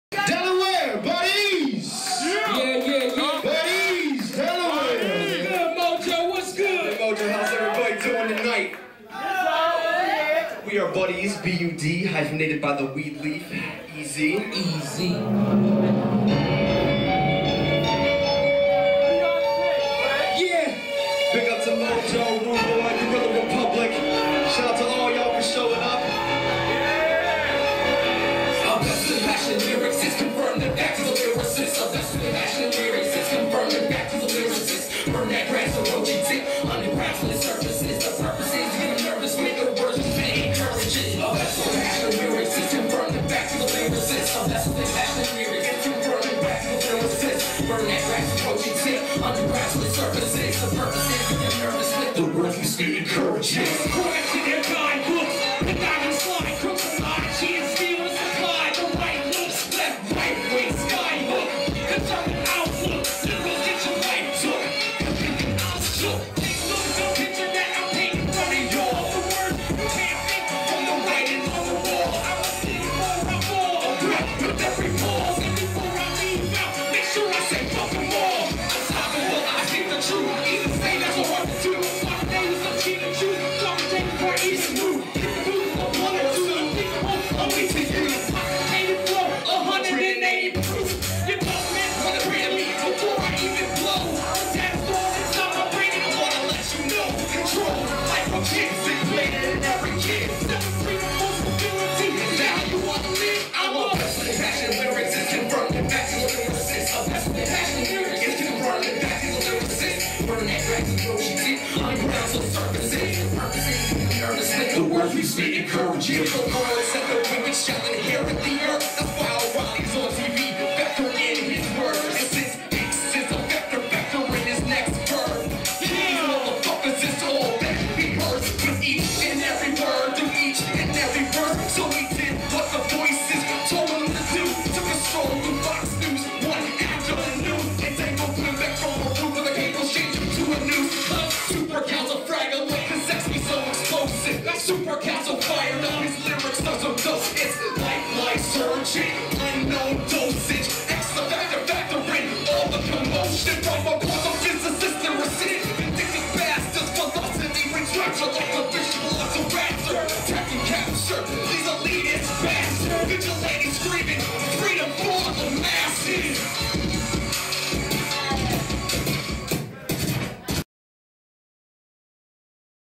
Bud-EZ performing @MOJO 13
Keep Delaware Hip-Hop Strong Event

Lyrics:

A vessel spittin passion in lyrics its/
Confirmed infact he's a lyricist/
Confirmed in fact he's a lyricist/
Burn that grass to the roachy tip/
Underground till it surfaces/
the purpose is, to get'em nevous with/
The words we spit encourages/ X2

Verse 1
Now According to their Guide Books Conniving sly crooks/
a lie cheat and steal to supply the right look/
left right wing, sky hook conservative outlook/
liberals a get your life took for thinkin i'm shook/
Now take a look at the picture that i'm paintin in front of ya'll/
the words you cant think from the writings on the wall/ imma spit'em before i fall a breath with every pause/
and before i leave out make sure i say fuck'em all/ unstoppable i speak the truth easy to say not so hard to do/
imma start the day with some gin and juice spark a J before i even move/
hit the booth with a blunt or two unthinkable only to you/ intoxicated flow 180 proof/
your government wanna ban me before i even blow/
for the data stored inside my brain they dont wanna let you know/
controlled micro-chips implanted in every kid/
sellin freedom for stability is that how you wanna live/...I'm

The words we spit encourages/

Verse 2
If the lord said the wicked shall inherit the earth/
thats why O'reilly's on T.V factoring in his word/
and since X is the factor, factor in his next verb,((KILL))
these Muthafuckaz is all that he heard/
with each and every word, through each and every verse/
So he did what the voices told him to do/
took a stroll through fox news one afternoon/
and dangled Glen Beck off the roof with a cable shape shift to a noose/
Cuz super-Cal-a-Frag-a-lick, cuz X be so explosive/
Got super Cal so Fired up his lyrics are so Dope its/
like lysergic unknown dosage/
X is the factor, factor in all the commocean/
from a quantum physicist lyricist
vindictive bastard/
my velocity brings rapture like a vicious velociraptor/
attack and capture these elitist bastard/
vigilante screamin freedom for the masses...